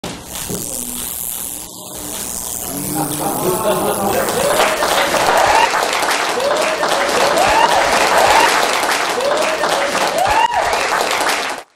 Lighting a light bulb using bare hands and a Tesla Coil!
Thom Britton, of FreakShow & Tell lights a bulb with his bare hands.